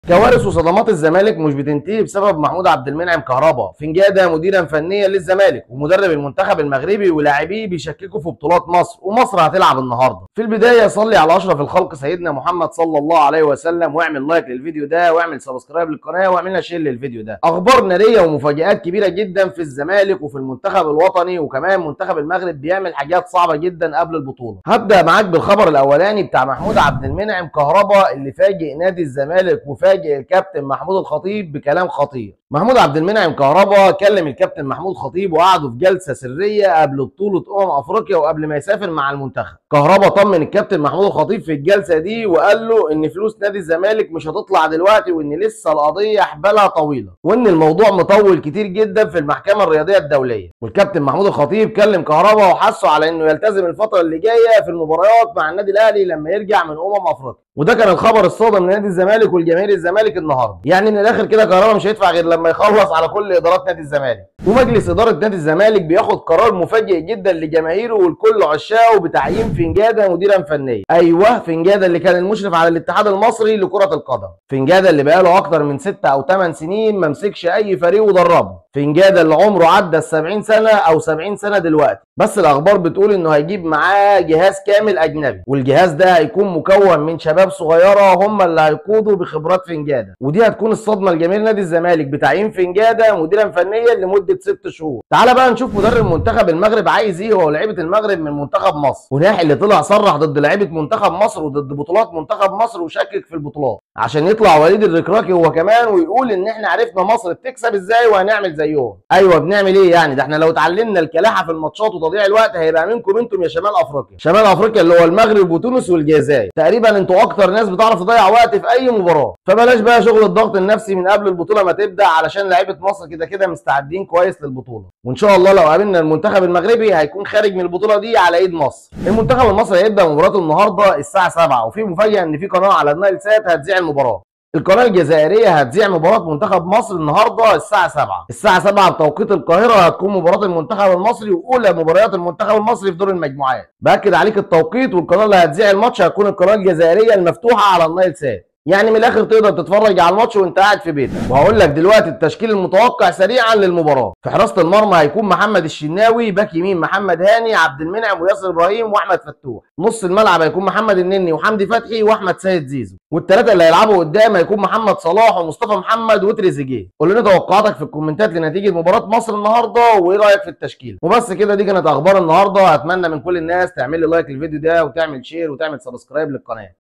تشكيلة منتخب مصر | كهربا بيعلم على الزمالك تانى | مدرب الزمالك الجديد رسما | ماتش مصرعلى قناه مجانيه
تشكيلة منتخب مصر | كهربا بيعلم على الزمالك تانى | مدرب الزمالك الجديد رسما | ماتش مصرعلى قناه مجانيه و كل اخبار الاهلى و الزمالك و الكره المصريه